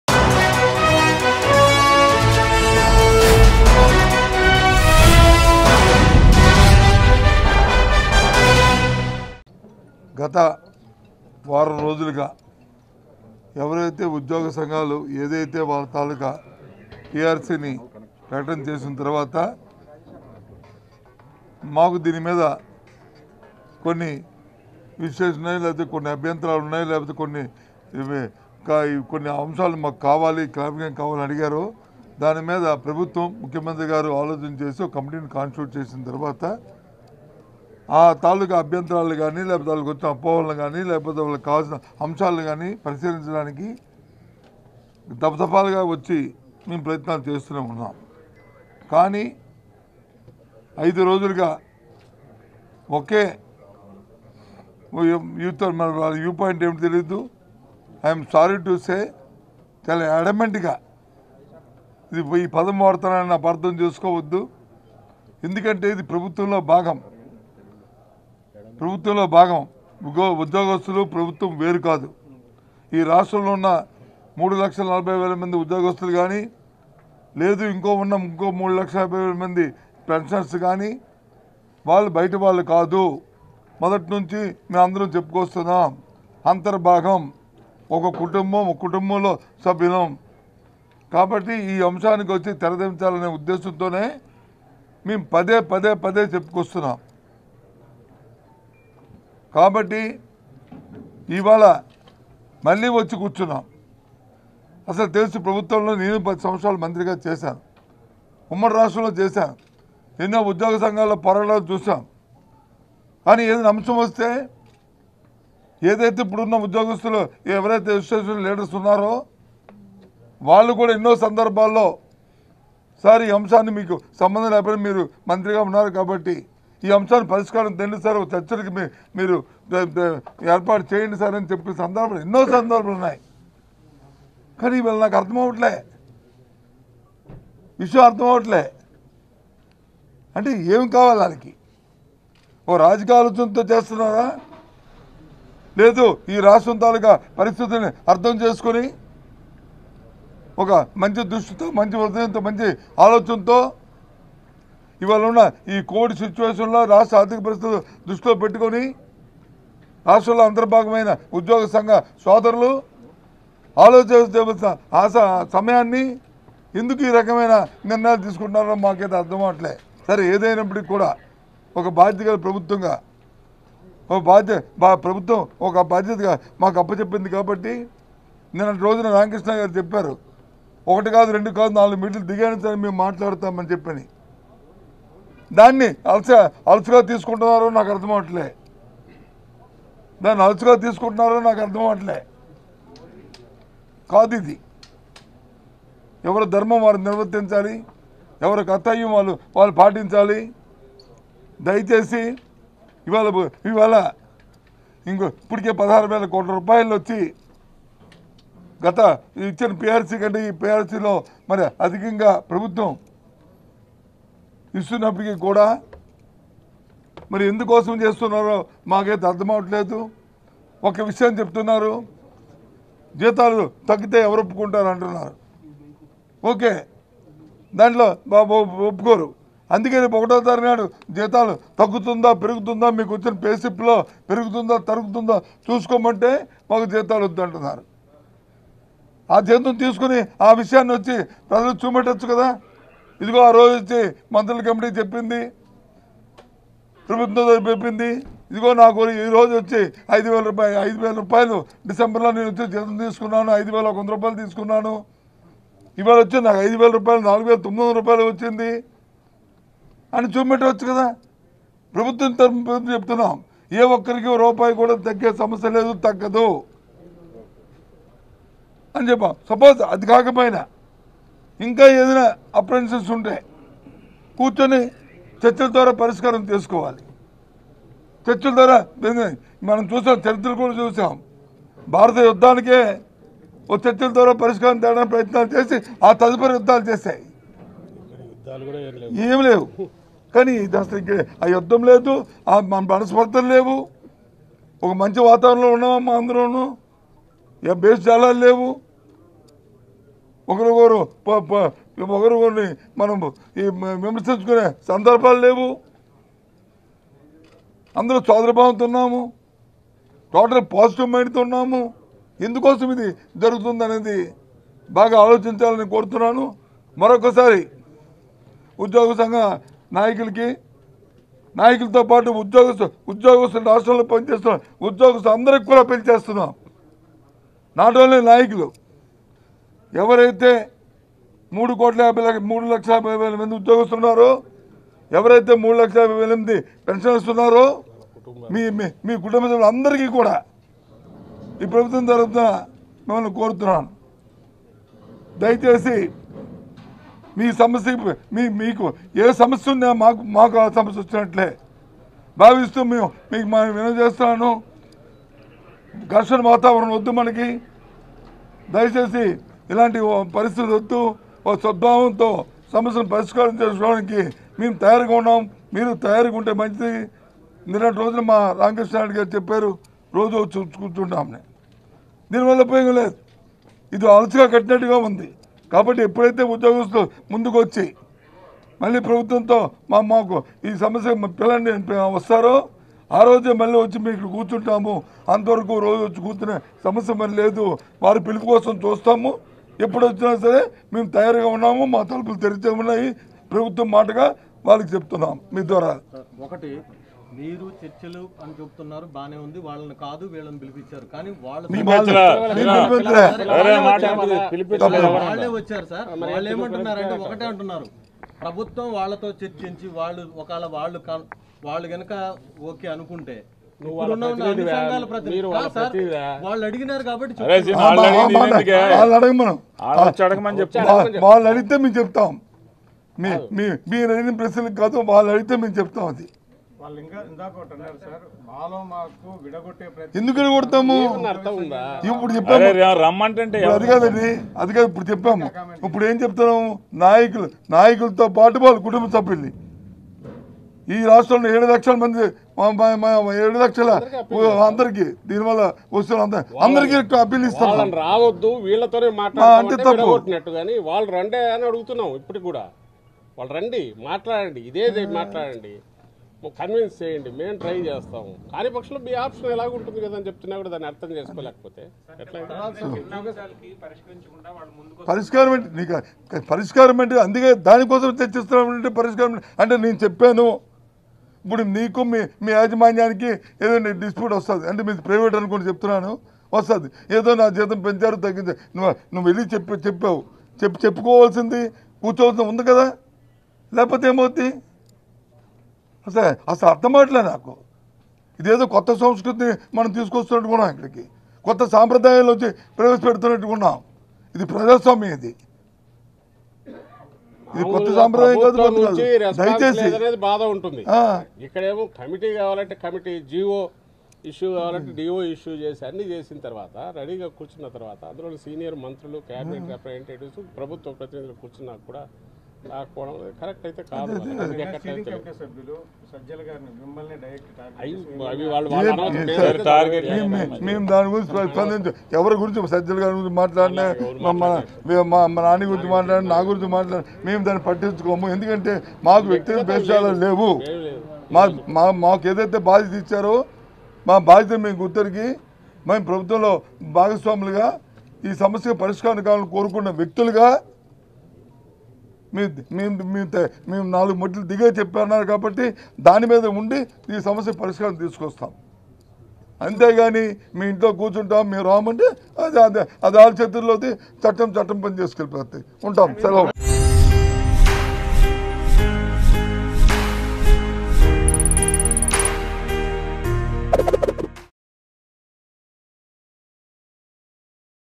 Minister Botsa Satyanarayana Sensational Comments On AP Govt Employees Protest | Mango News
Minister Botsa Satyanarayana Sensational Comments On AP Govt Employees Protest.